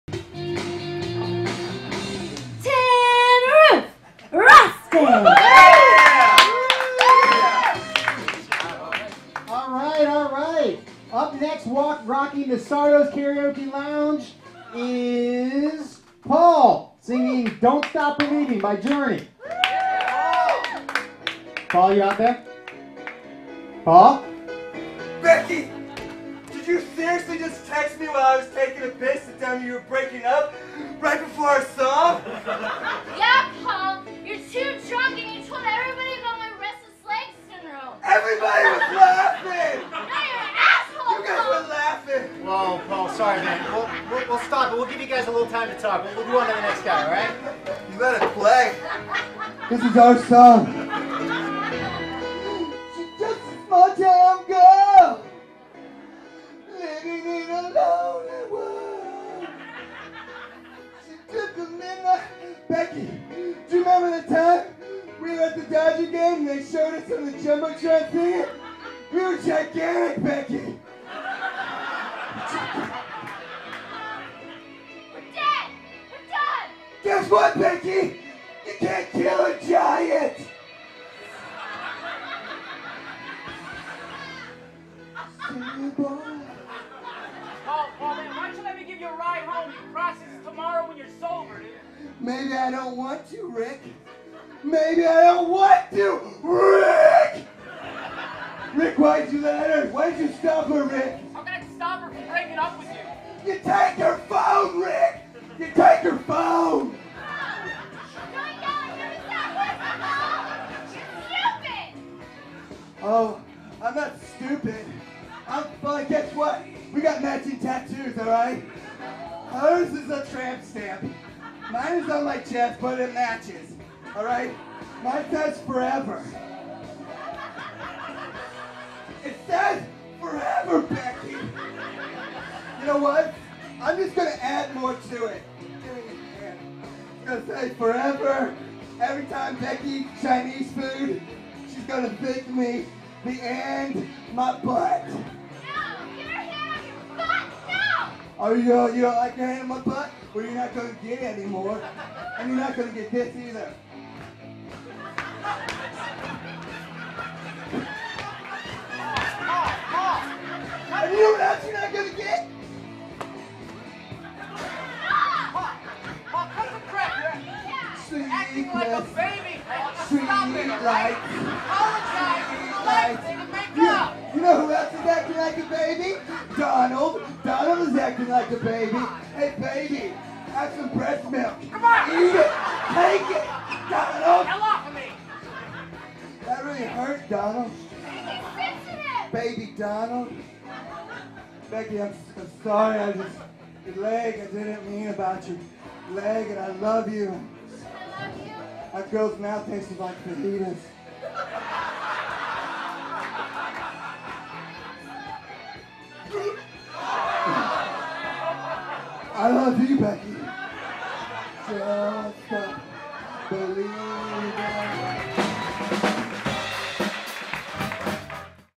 Do Stop Believin'
Sketch written and performed  by Tom Burkhardt at The Groundlings Theater.  Also starring Patty Guggenheim, Harmony McElligott, Rob Vestal, Kimberly Condict, Amanda Ohly